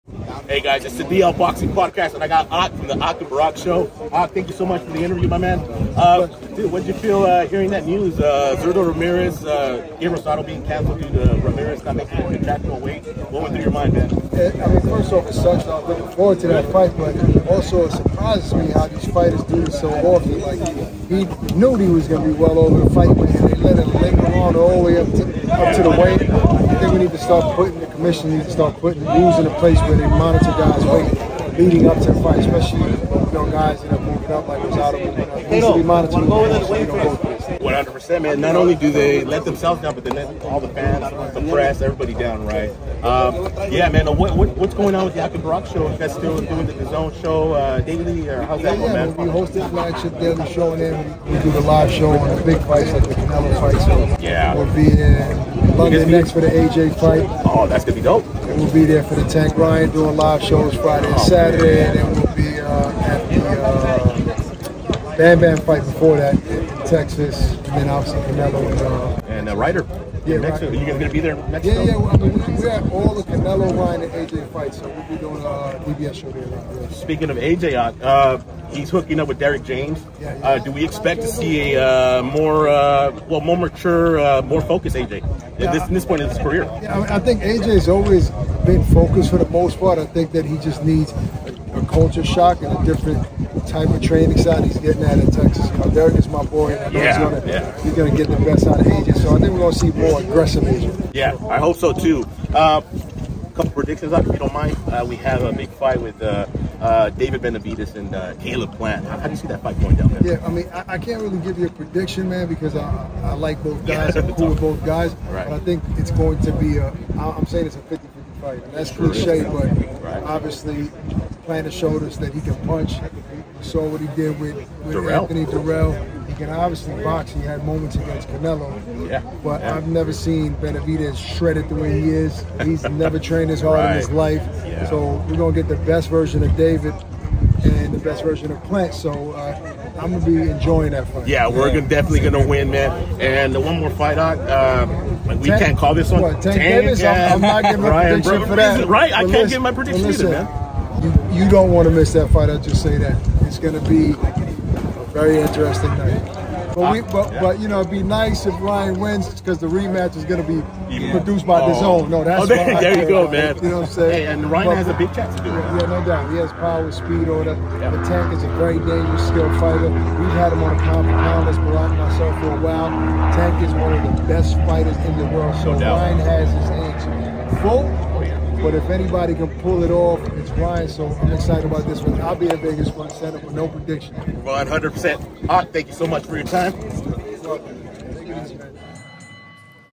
DL Boxing Podcast interview with boxing analyst Ak, talking about the sport you and I love, Boxing!
DL Boxing Podcast chops it up with boxing analyst Ak of the DAZN Boxing Show. We talk about the sport you and I love, Boxing! It was great to meet such a candid professional sports personality!